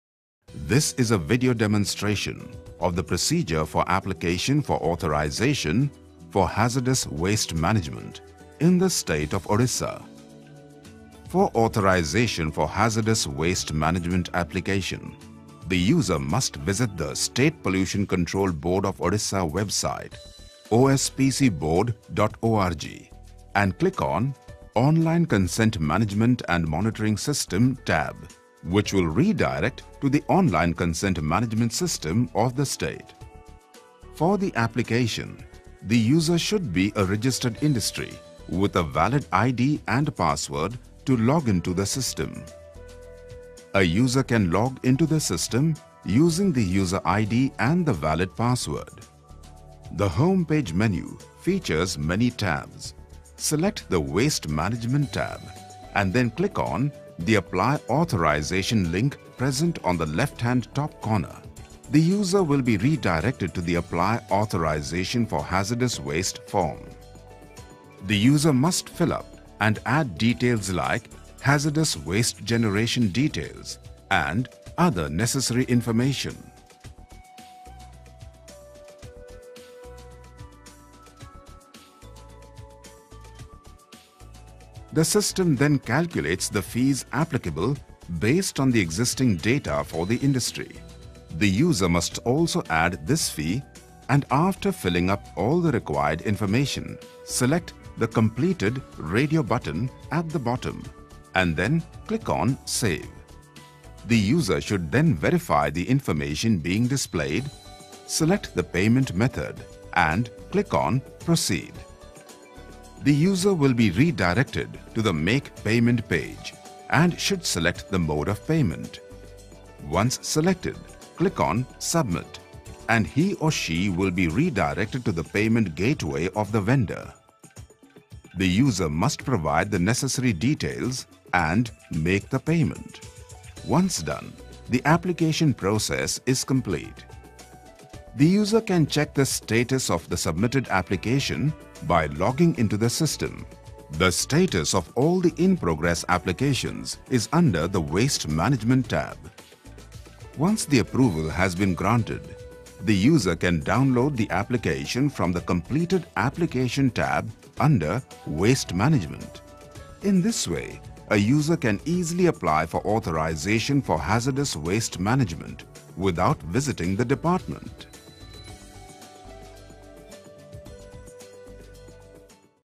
Authorization for Hazardous Waste Management
Video Demonstration for procedure of Authorization for Hazardous Waste Management